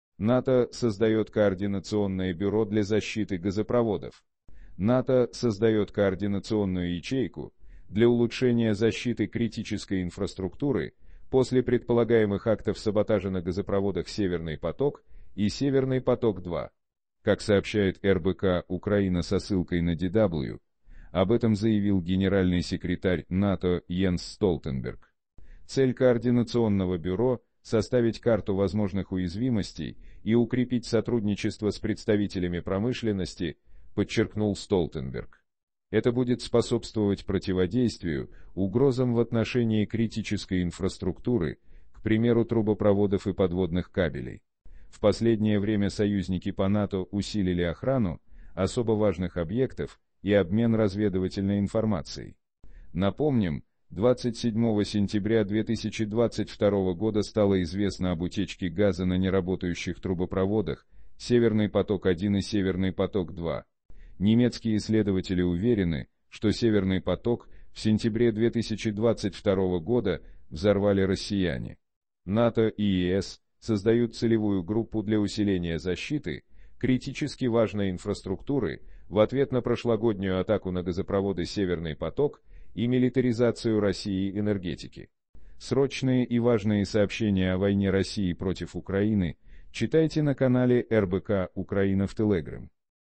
НАТО создает координационное бюро для защиты газопроводов.
НАТО Северныйпоток-2 НАТО создает координационную ячейку для улучшения защиты критической инфраструктуры после предполагаемых актов саботажа на газопроводах "Северный поток" и "Северный поток-2". Как сообщает РБК-Украина со ссылкой на DW, об этом заявил генеральный секретарь НАТО Йенс Столтенберг. Цель координационного бюро - составить карту возможных уязвимостей и укрепить сотрудничество с представителями промышленности, подчеркнул Столтенберг. Это будет способствовать противодействию угрозам в отношении критической и